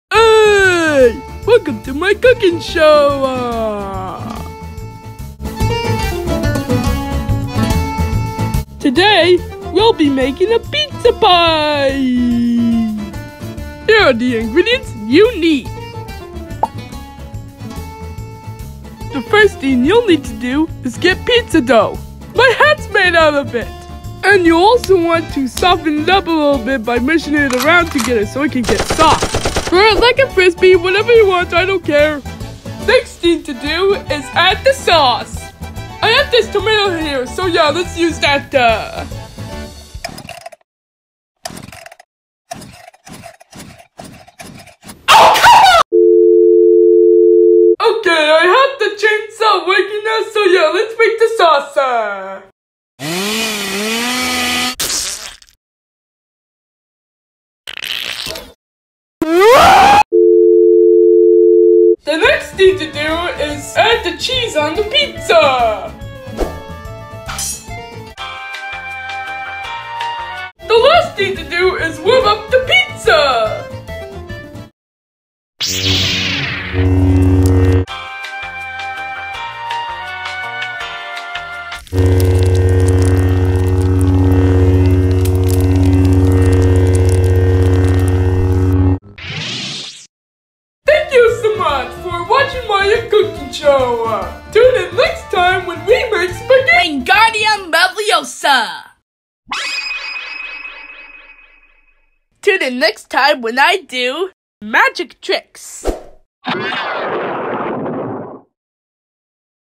Chef Luigi’s Cooking Show Episode 1: How To Make A Pizza Pie
Chef Luigi Shows You How To Make Pizza.